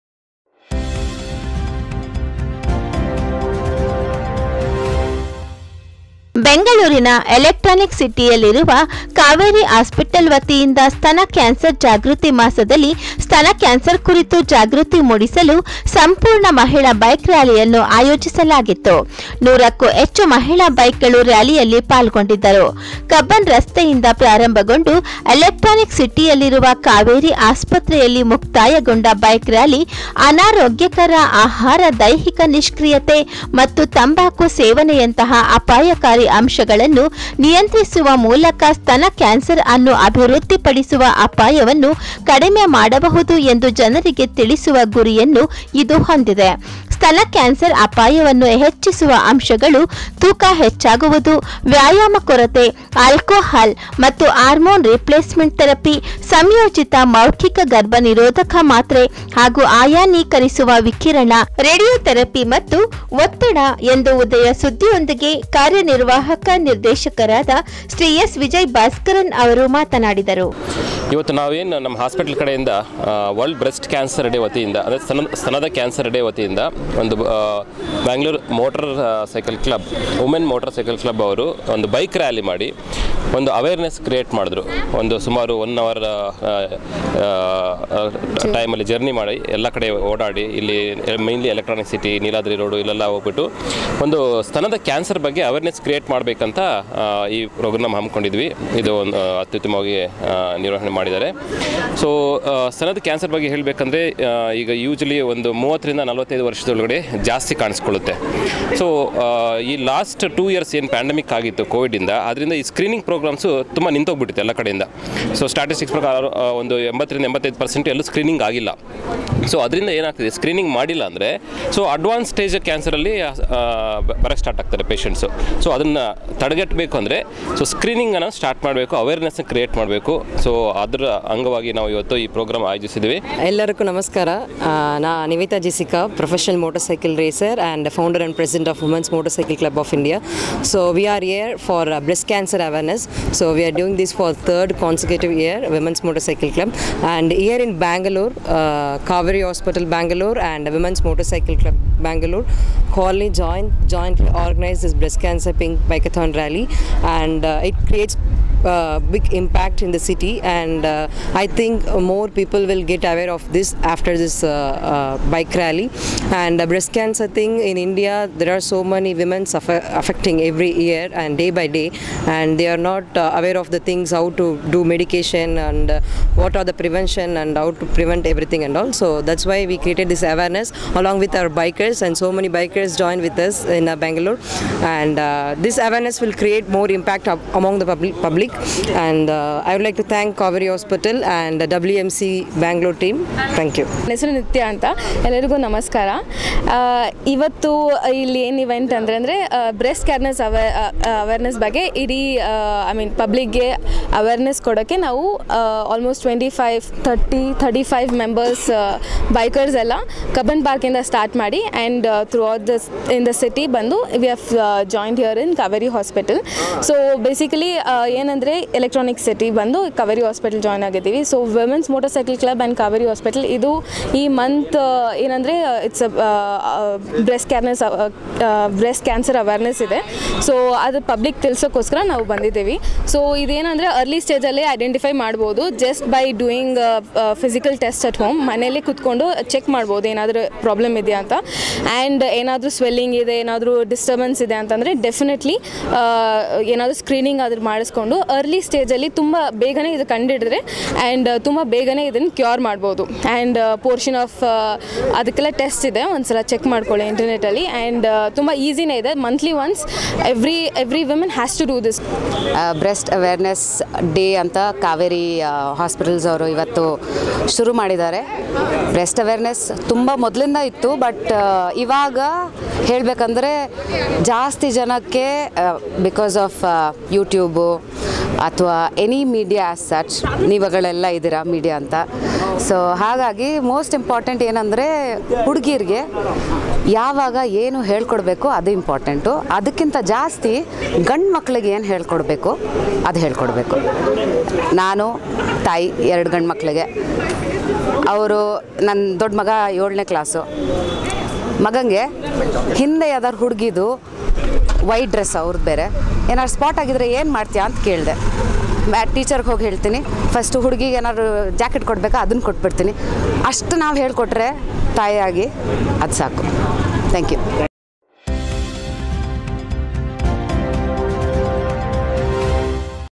ಕಾವೇರಿ ಹಾಸ್ಪಿಟಲ್ ವತಿಯಿಂದ ಸ್ತನ ಕ್ಯಾನ್ಸರ್ ಕುರಿತು ಜಾಗೃತಿ ಮೂಡಿಸಲು ಸಂಪೂರ್ಣ ಮಹಿಳಾ ಬೈಕ್ ರ್ಯಾಲಿ
KAUVARY HOSPITAL BREAT CENCER AWERENESS